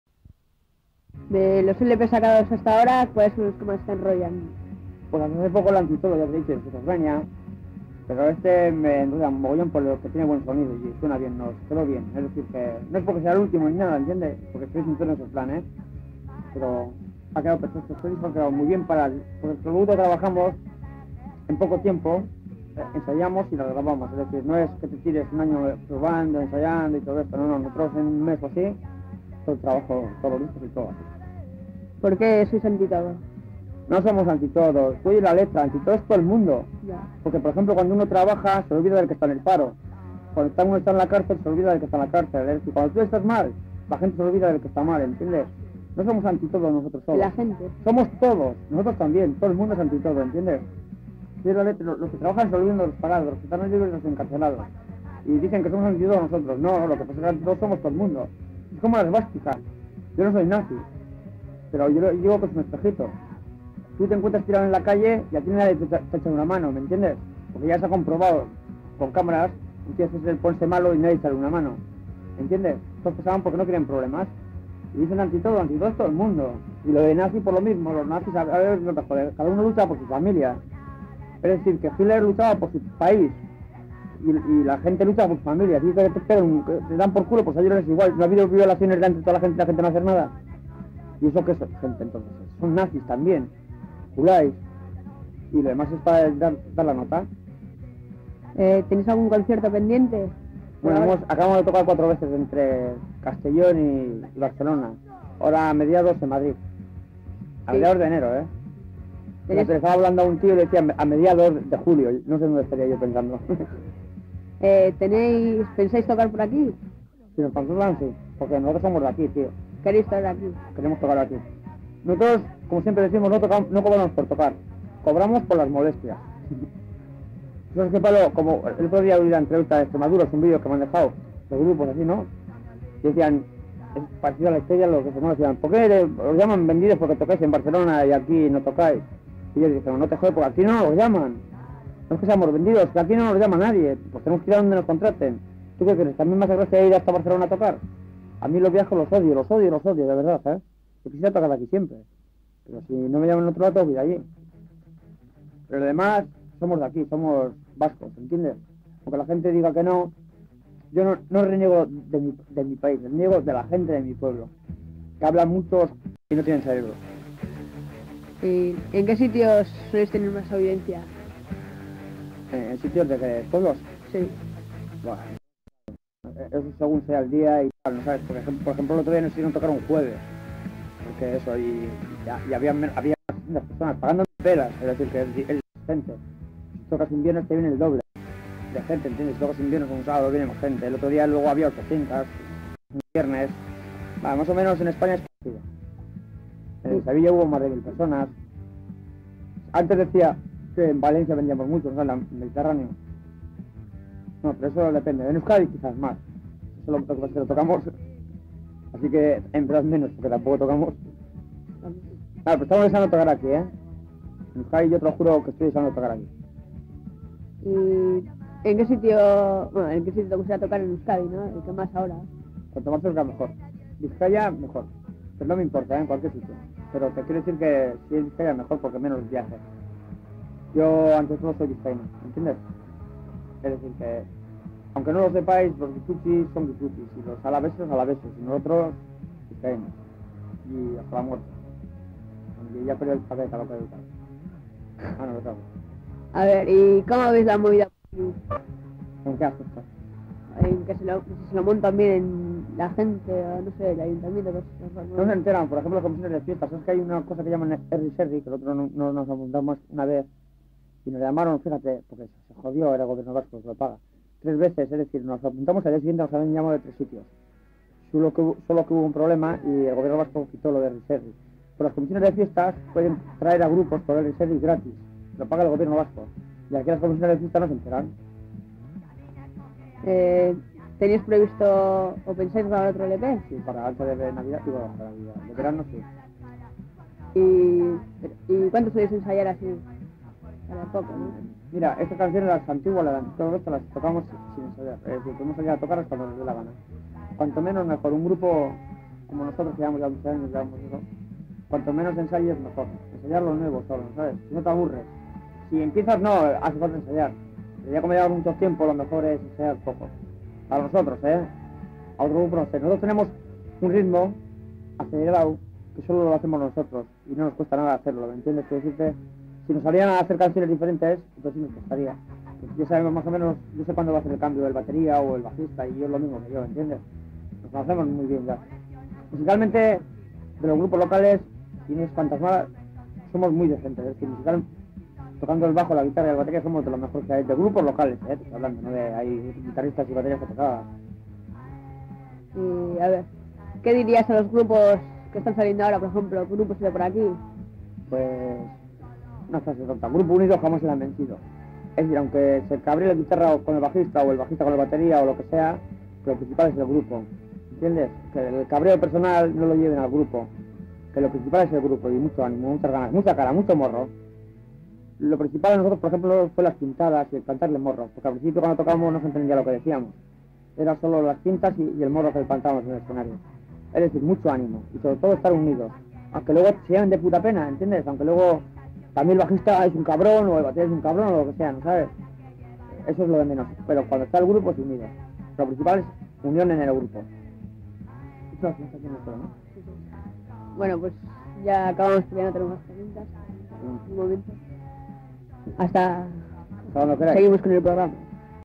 Entrevista a Iosu Eskorbuto 1991
Entrevista a Iosu Exposito [Eskorbuto] en el año 1991 realizada en Pititako Irratia, Santurtzi. Realizada por Javi, el cantante de Subversion x

Esta grabación pertenece a los archivos de Juan Carlos Azkoitia. Puedes contactar con el en su facebook